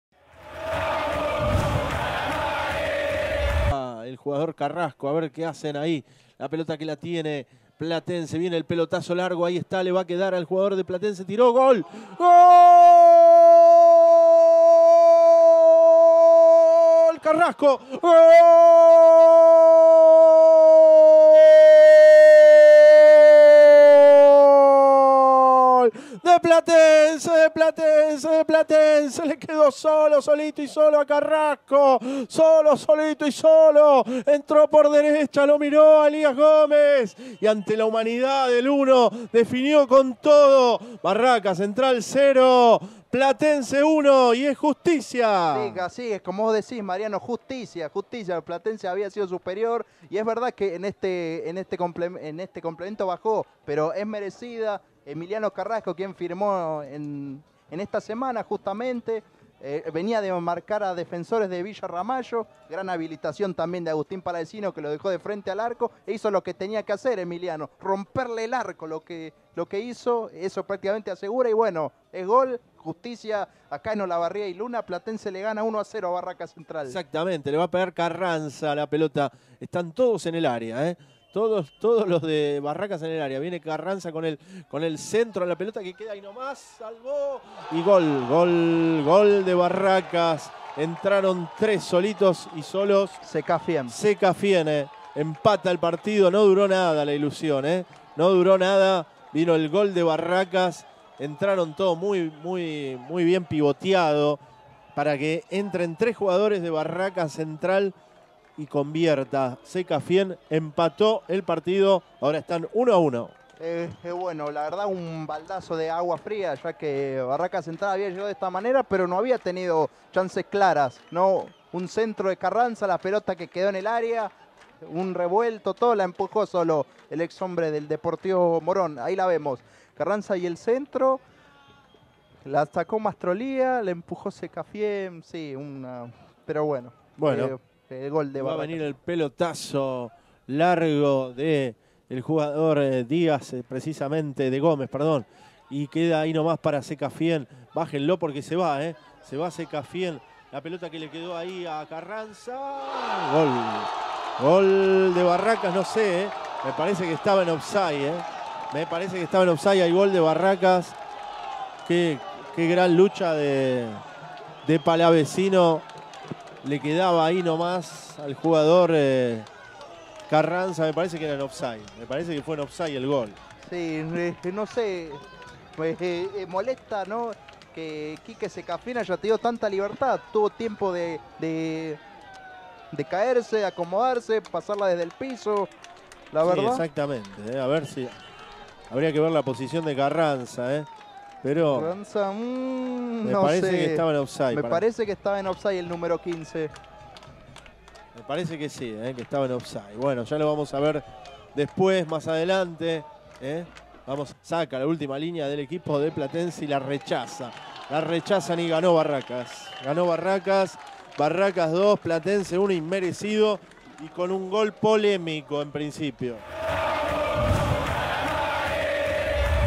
Barracas Central 2 - 1 Platense | Fecha 01 | Campeonato 2016/2017
Compacto con los goles y las jugadas más destacadas de la derrota de Platense por 2-1 ante Barracas Central, por el partido correspondiente a la 1ª jornada del Campeonato 2016/2017 de la Primera "B" Metropolitana, en el estadio Claudio Tapia. Arbitraje de Paulo Vigliano.